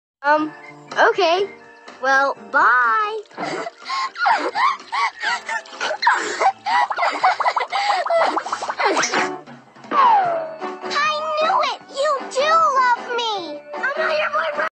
Gacha life drarry/harco skit IM NOT YOUR BOYFRIEND
Sorry this took a while to get out 😅😓